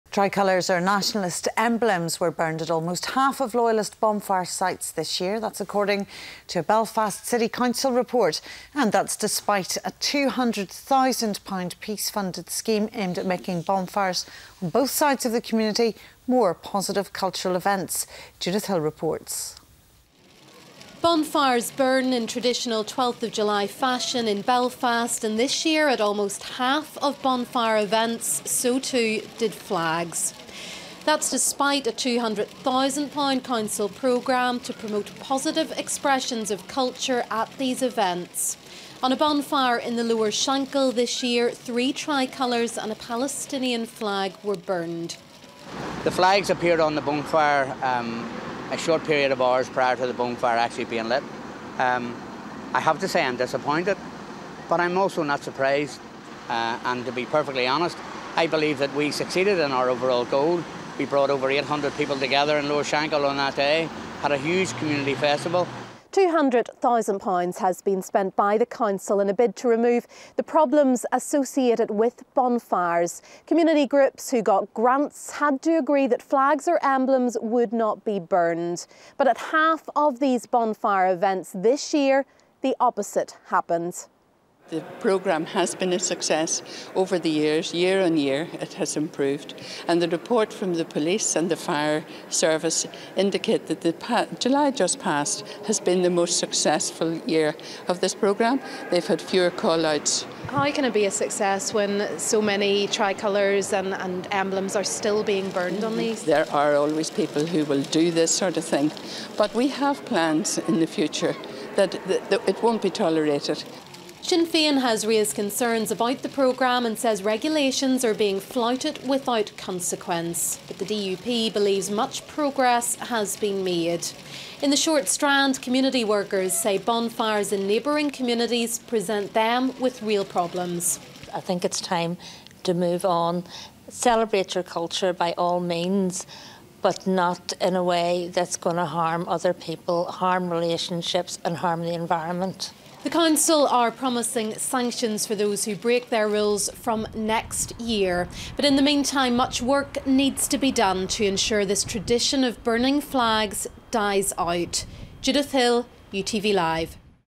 Irish flag burned on more than half of Loyalist Bonfires
We hope to see an end to flag burning and these Bonfires. They only continue separation of communities and breed sectarianism. They are given £200,000 each year with the condition that there will be no flag burning so that it is not offensive. A waste of money for a sectarian festival. Spend that money on a Children's Hospital, not a sectarian party!

Tricolours or nationalist emblems were burned at almost half of loyalist bonfire sites this year, according to a Belfast City Council report.

It comes despite a £200,000 scheme aimed at making such events on both sides of the community more positive cultural expressions.

Last summer, one bonfire on the Lower Shankill in west Belfast was lit after being adorned with three tricolours and a Palestinian flag.

Ian McLaughlin, from the Lower Shankill Community Association, said: "The flags appeared on the bonfire in a short period of hours prior to the bonfire being lit.

"I have to say I am disappointed, but I am also not surprised and, to be perfectly honest, I believe that we succeeded in our overall goal, we brought over 800 people together in Lower SHankill on that day. We had a huge community festival."

The money spent by the council was issued to community groups who had to agree that flags or emblems would not be burned. But at half of these events the opposite happened.

Nevertheless, Belfast City Council insists the programme is paying dividends

Máire Hendron, who chairs the council's Good Relations Partnership, said: "The program has been a success over the years - year on year it has improved.

"The report from the police and the Fire Service indicate that the July just passed has been the most successful year of this program. They had fewer call outs. There are always people who will do this sort of thing, but we have plans in the future that will not be tolerated."

Sinn Féin has raised concerns about the programme and said regulations are being flouted without consequence, while the DUP believes much progress has been made.

In the Short Strand in east Belfast, community workers said bonfires in neighbouring communities present them with real problems.

Patricia Johnson, of the Short Strand Partnership, said: "I think it is time to move on - celebrate your culture by all means but not in a way that will harm other people."

Meanwhile, the council is promising sanctions for those who flout their rules next year.